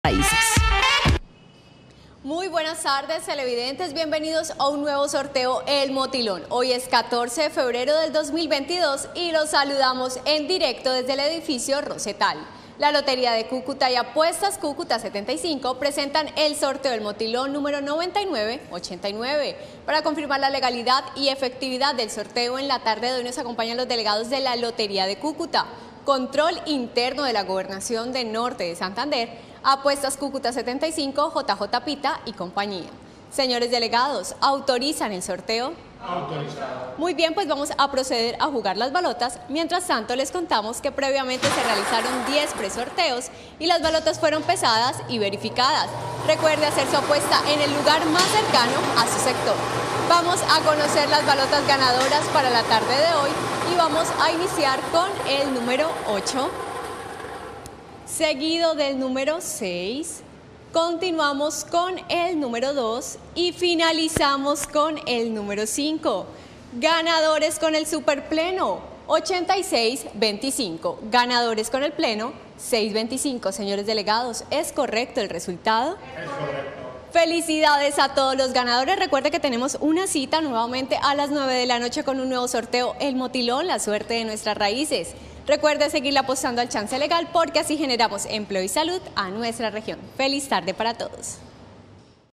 MOTILÓN TARDE Resultado del LUNES 14 de febrero de 2022 ✅🥇🔥💰
Hola buenas tardes, te presentamos los resultados de las loterías y chances en Colombia, vamos con el Resultados del Chance Motilón tarde del LUNES 14 de febrero de 2022.